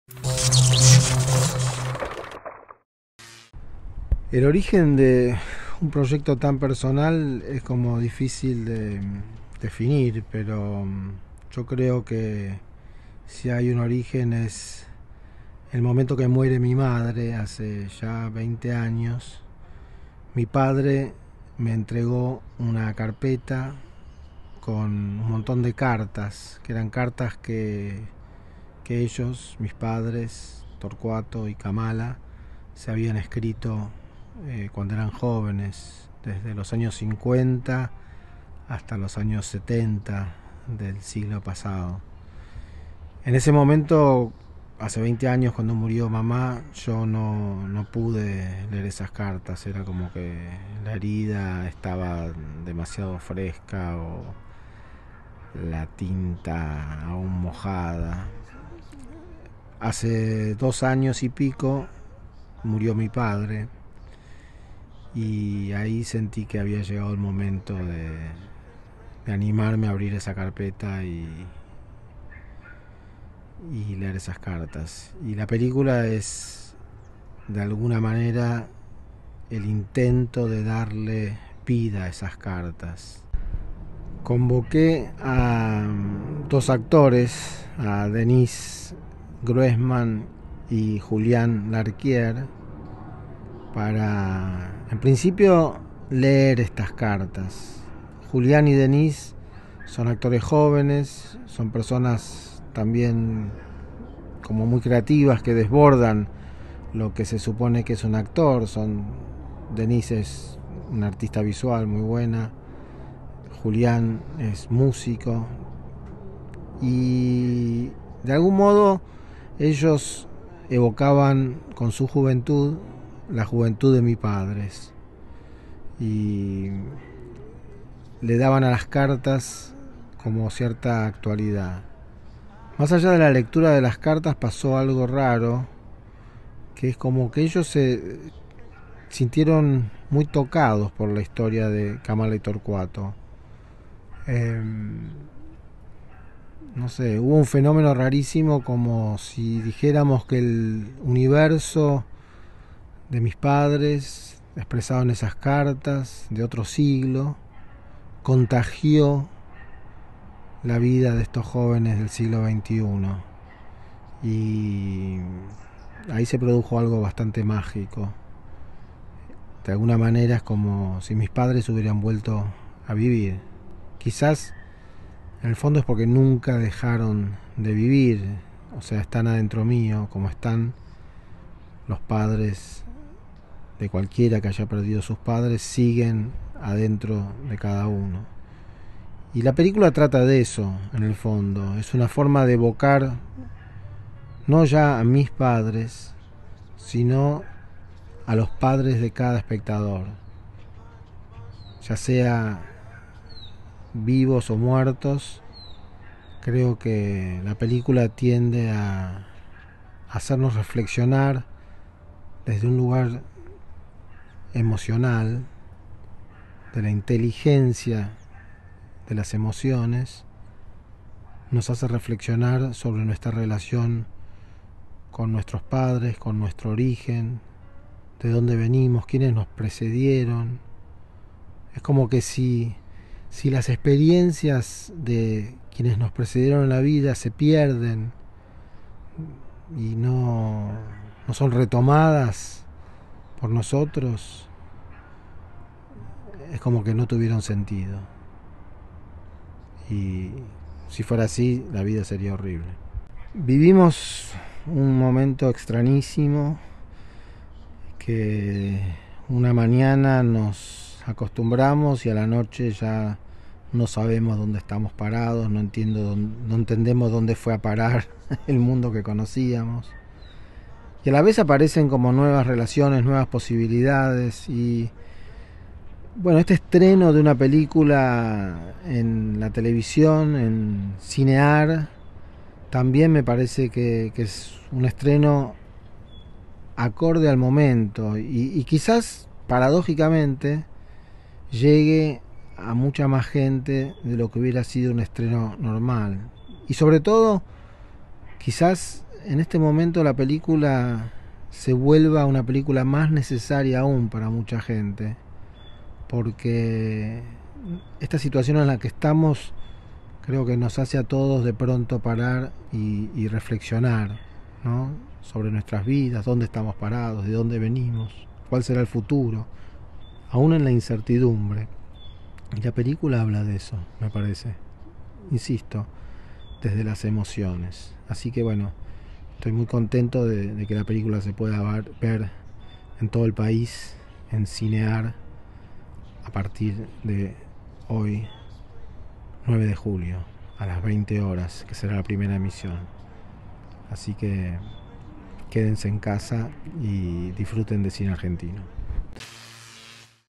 "Ficción privada": un filme sobre la familia Di Tella
Se trata del nuevo documental del director Andrés Di Tella, que tuvo su estreno por la pantalla de CINE.AR. Una película inspirada en las cartas que Torcuato entregó a su hijo.

Un actor y una actriz leen, durante varios días y noches, la correspondencia entre Torcuato y Kamala, los padres del director de la película. Él argentino, ella hindú. Las cartas atraviesan las décadas del 50 al 70, registran viajes por el mundo, hablan de amor e idealismo, de dolor y sueños rotos. Una historia privada del siglo veinte.

"En el momento que muere mi madre hace ya veinte años, mi padre me entregó una carpeta con cartas que mis padres se habían escrito cuando eran jóvenes, de los años 50 hasta los 70 del siglo pasado" dijo Andrés Di Tella.

"Hace dos años que murió mi padre, y entonces sentí que era el momento de animarme a  abrir esa carpeta y leer esas cartas, y la película es el intento de querer darles vida a esas cartas" cerró el director para Barricada TV.

Un historia de amor que viajó de generación a generación esperando ser contada, y en donde el realizador se convierte en un protagonista y lo documental se convierte en ficción.